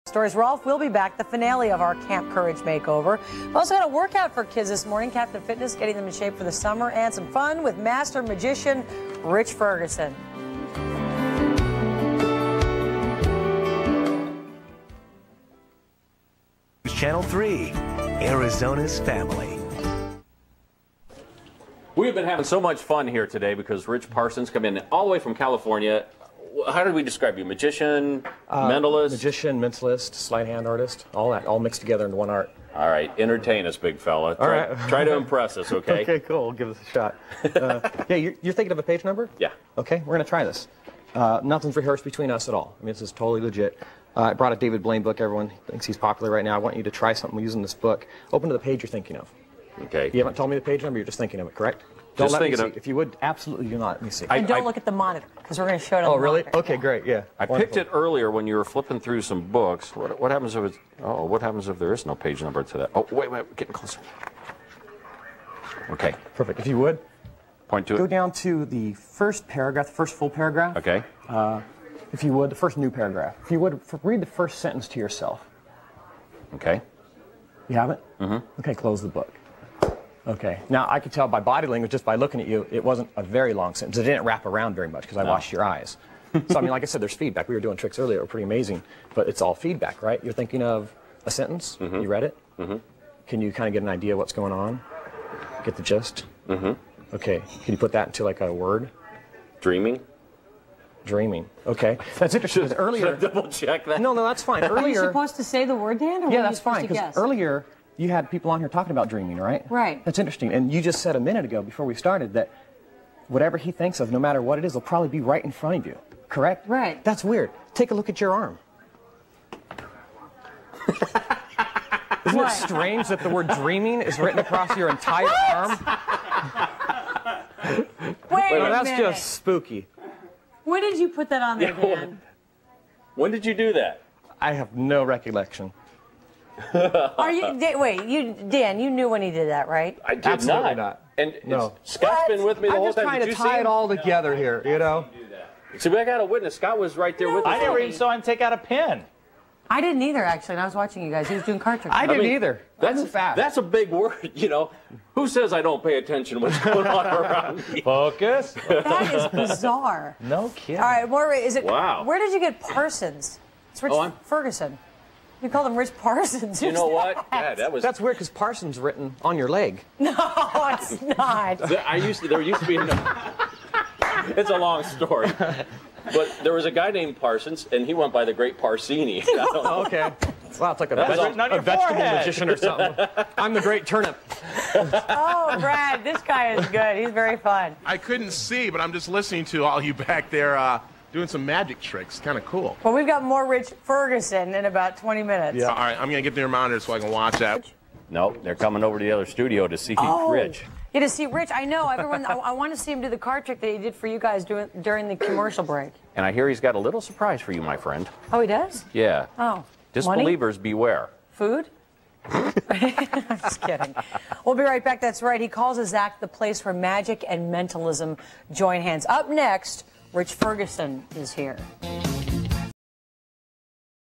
Magician Interviewed on TV does CRAZY Magic!
Magician Rich Ferguson has a guy read a sentense from book and make up a word the describes it... and that word appears on his freaking arm!.. live TV...